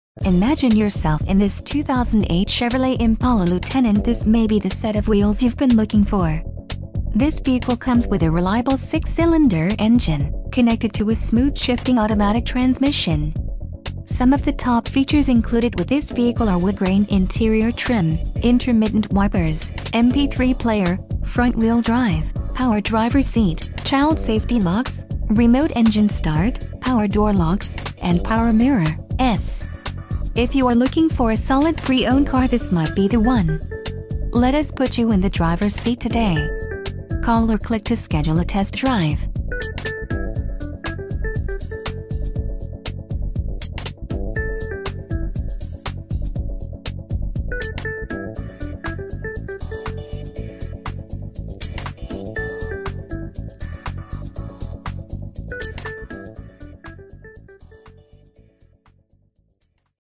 2008 Chevrolet Impala - TDR Auto Plaza - Kearney, MO 64060
GAS SAVER, LOCAL TRADE, DUAL CLIMATE CONTROL, CARFAX 1 owner and buyback guarantee** This Sedan has less than 61k miles*** How comforting is it knowing you are always prepared with this tried-and-trued LT w/3.5L* Priced below KBB Retail!!! Why pay more for less... All the right toys!!! Safety Features Include: Curtain airbags, Passenger Airbag, Daytime running lights, Dusk sensing headlights...Comes equipped with all the standard amenities for your driving pleasure: Power locks, Power windows, Auto, Air conditioning, Front air conditioning zones - Dual...